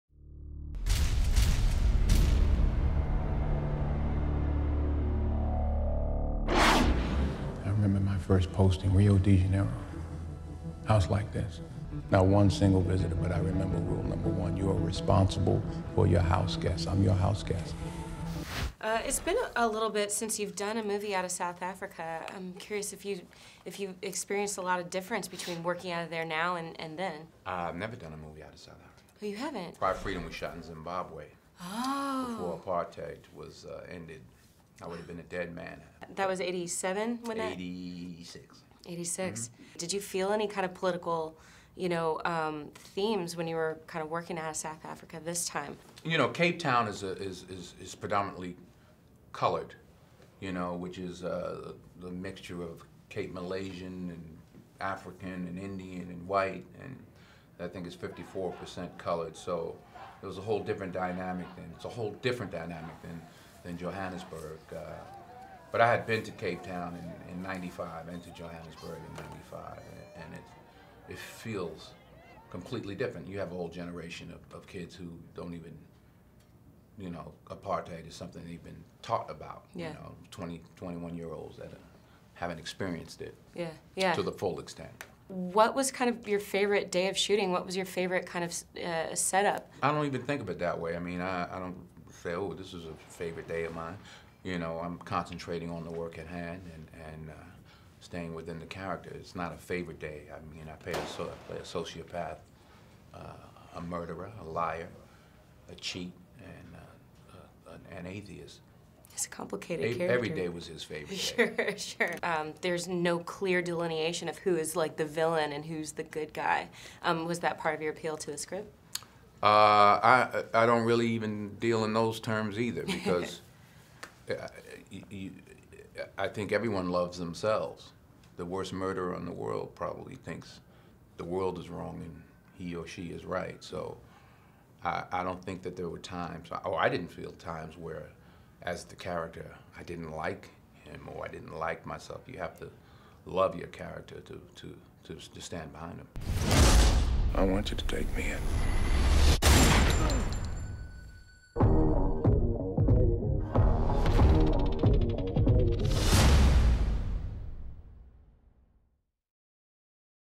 Denzel Washington talks how he stayed in character on the set of 'Safe House'
Katie Hasty of HitFix talks to Denzel Washington about his new film 'Safe House' and about his character, "Tobin Frost." Mr Washington claims he had no good days or bad on set, but just remained in character as a psychopath.